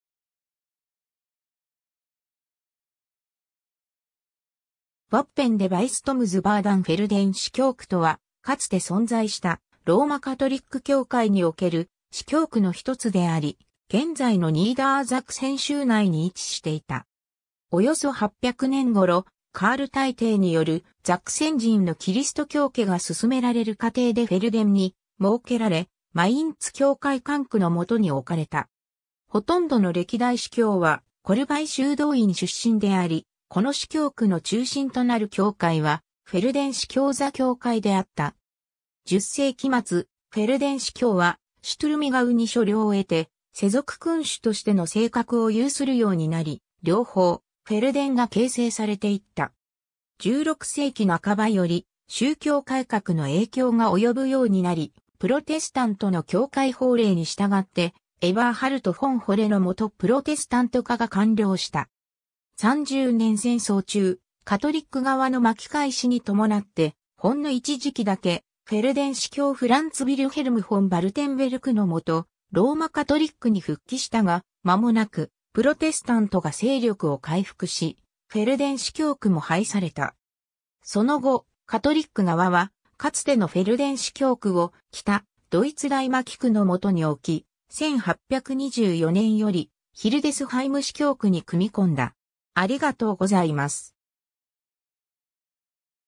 フェルデン司教区
Wappen des Bistums Verden フェルデン司教区 (Bistum Verden) とは、かつて存在したローマ・カトリック教会における司教区の一つであり、現在のニーダーザクセン州内に位置していた。
 およそ800年頃、カール大帝によるザクセン人のキリスト教化が進められる過程でフェルデンに設けられ、マインツ教会管区のもとに置かれた。
ほとんどの歴代司教はコルヴァイ修道院出身であり、この司教区の中心となる教会はフェルデン司教座教会であった。
10世紀末、フェルデン司教はシュトゥルミガウに所領を得て世俗君主としての性格を有するようになり、領邦フェルデンが形成されていった。
16世紀半ばより宗教改革の影響が及ぶようになり、プロテスタントの教会法令に従って、エバーハルト・フォン・ホレ (Eberhard von Holle wurde) のもとプロテスタント化が完了した。
三十年戦争中、 カトリック側の巻き返しに伴って、ほんの一時期だけフェルデン司教フランツ・ヴィルヘルム・フォン・ヴァルテンベルク (Franz Wilhelm von Wartenberg) のもとローマ・カトリックに復帰したが、まもなくプロテスタントが勢力を回復し、フェルデン司教区も廃された。
その後、カトリック側はかつてのフェルデン司教区を「北ドイツ代牧区」 (Apostolische Vikariat des Nordens) のもとに置き、1824年よりヒルデスハイム司教区に組み込んだ。